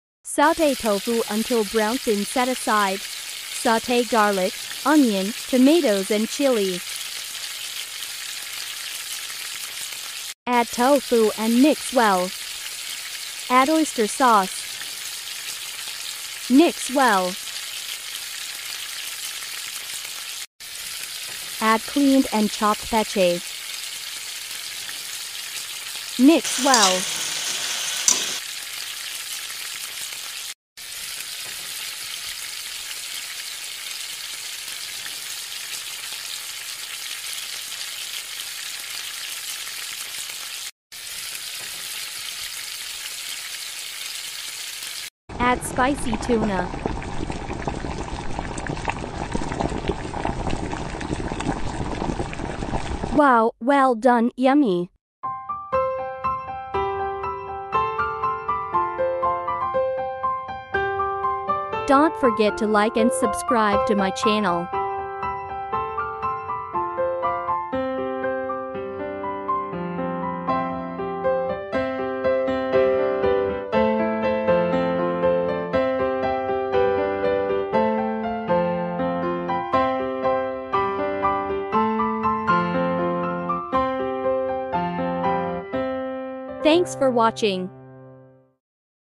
HOW TO COOK SPICY TOFU AND TUNA WITH PECHAY
INGREDIENTS

3 pcs tofu sliced into cubes
1tbsp minced garlic
1 pc onion (chopped)
1 green chili pepper
2 tbsp of oyster sauce
1 cup of spicy tuna flakes
   salt
   black pepper
2 cups of chopped pechay

Always kiss the cook 💋